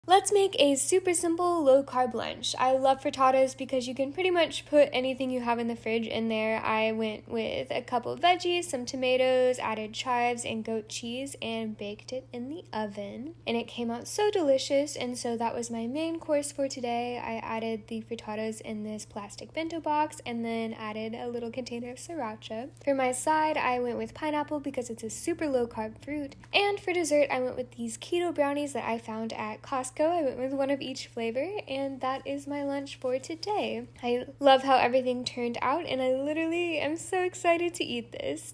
Low Carb Frittata
I love making frittatas because they are low-carb and very low-effort and are a great way to clean out the fridge. ✨

Here is how you make it:
Start with whatever veggies you have in your fridge! I went with onion, tomato, bell pepper, and yellow squash. 🧅🥚🫑🍅

Saute your veggies until tender. I like to use a cast iron pan to transfer them easily to the oven. Once your veggies are sauteed, turn off the heat, and add 6-8 whisked eggs. Stir until veggies are combined.

I added chives and goat cheese at this point, but feel free to add whatever you have on hand!

Bake in the oven at 350ºF for 8-10 minutes, broiling for the last few minutes, so the top finishes cooking.

And you’re finished!